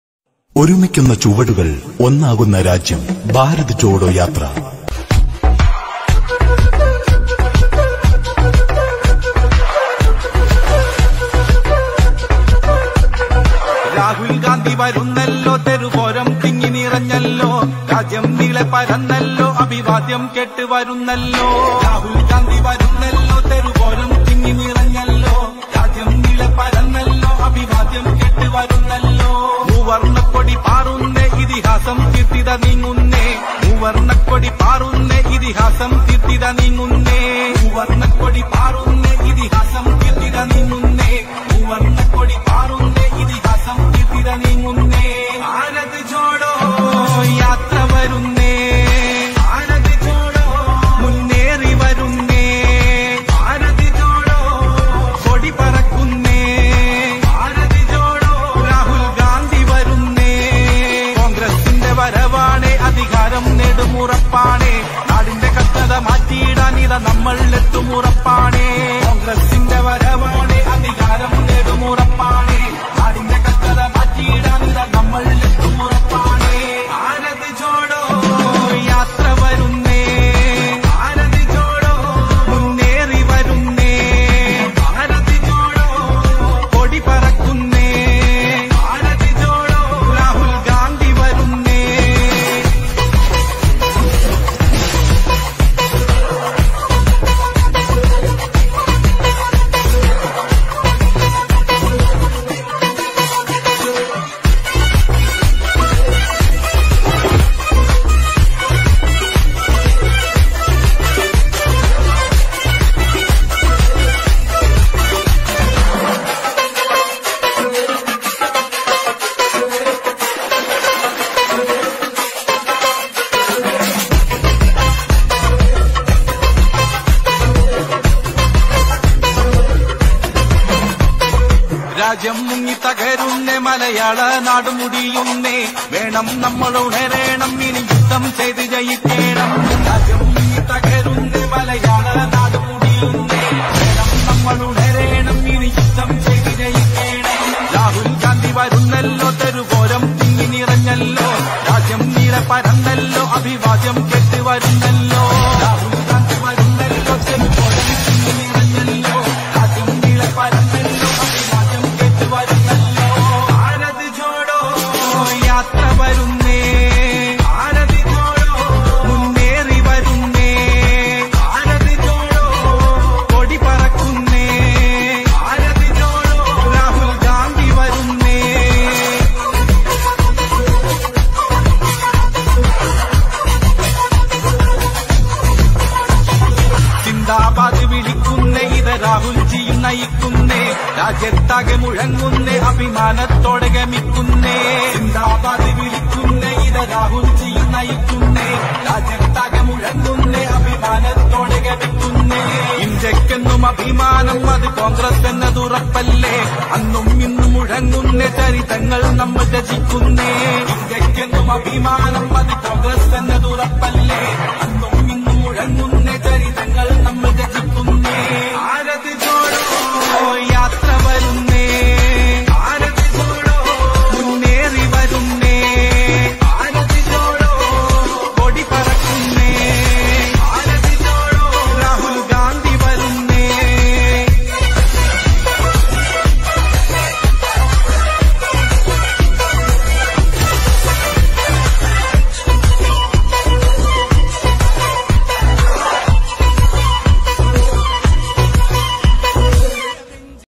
BHARAT JODO YATRA_വാഹന പ്രചരണ റാലി.@ചെത്ത്ലത്ത് ദ്വിപ്-Bharat jodo yatra
"ഒരുമിക്കുന്ന ചുവടുകൾ-ഒന്നാകുന്ന രാജ്യം " എന്ന മഹത്തായ മുദ്രാവാക്യം ഉയർത്തിക്കൊണ്ട് രാഹൂൽഗാന്ധി നയിക്കുന്ന ഭാരത ജോഡോ യാത്രയുടെ പ്രചരണാർത്ഥം ലക്ഷദ്വീപിലെ ചെത്ത്ലത്ത് യൂണീറ്റ് ബ്ലോക്ക് കോൺഗ്രസ്സ് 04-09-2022 ന് സംഘാടിപ്പിച്ച വാഹന റാലി.